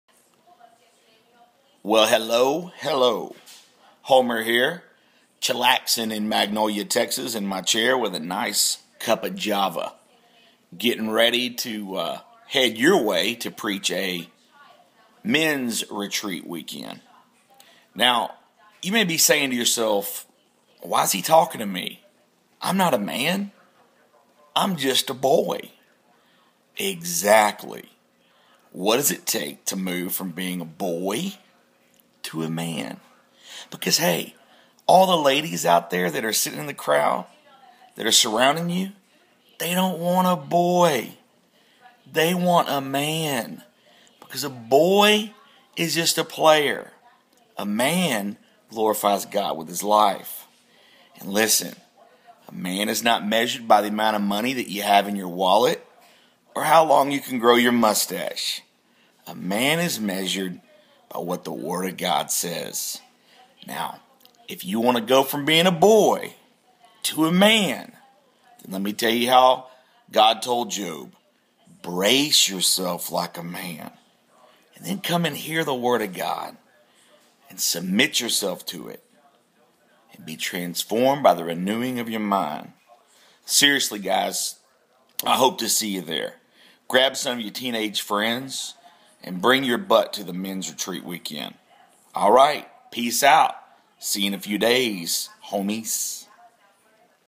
Don't be a Boy
The measure of a man is more than the thickness of his wallet or the length of his mustache. Are you a man, as defined by The Word of God? Well, let's see...